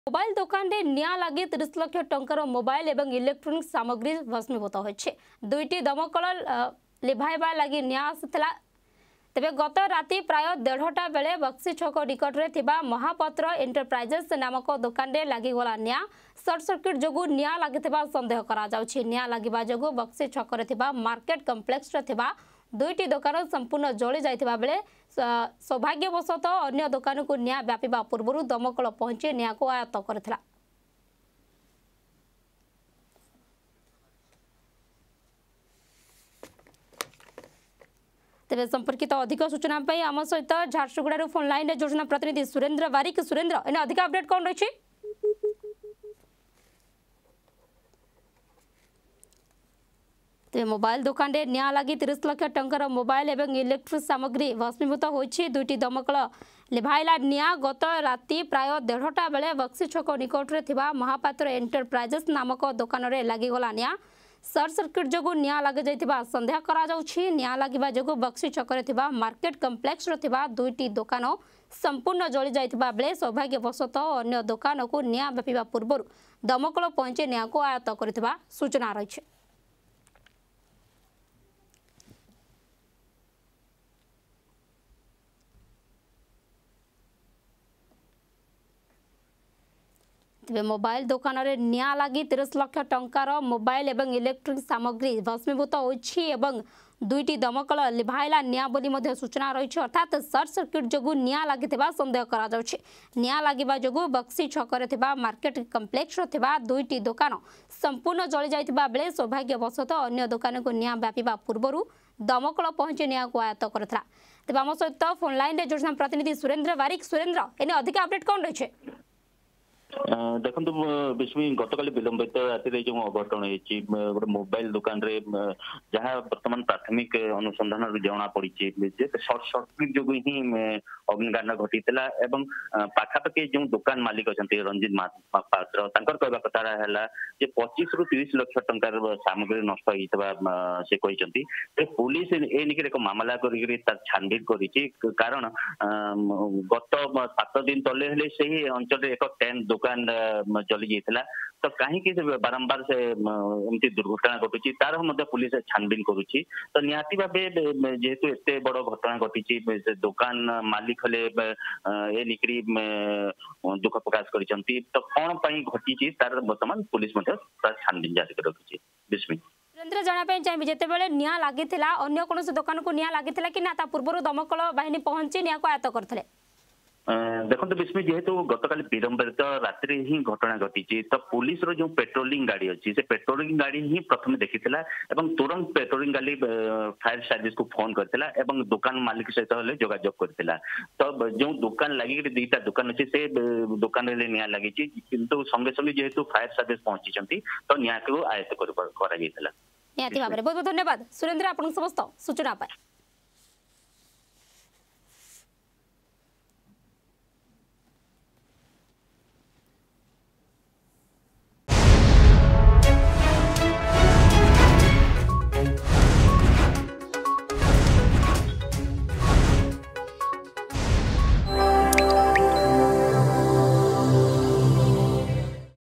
ମୋବାଇଲ ଦୋକାନରେ ନିଆଁ
ମୋବାଇଲ ଦୋକାନରେ ନିଆଁ ଲାଗି 30 ଲକ୍ଷ ଟଙ୍କାର ମୋବାଇଲ ଓ ଇଲେକ୍ଟ୍ରୋନିକ୍ସ ସାମଗ୍ରୀ ଭସ୍ମୀଭୂତ ।
ଗତ ରାତି ପ୍ରାୟ ଦେଢ଼ଟାବେଳେ ବକ୍ସି ଛକ ନିକଟରେ ଥିବା ମହାପାତ୍ର ଏଣ୍ଟରପ୍ରାଇଜେସ ନାମକ ଦୋକାନରେ ନିଆଁ ଲାଗିଯାଇଥିଲା । ସଟ ସର୍କିଟ ଯୋଗୁଁ ନିଆଁ ଲାଗିଥିବା ସନ୍ଦେହ କରାଯାଉଛି...
ଅଧିକ ଖବର ପାଇଁ ଡ଼ାଉନଲୋଡ କରନ୍ତୁ “Argus News” ଆପ୍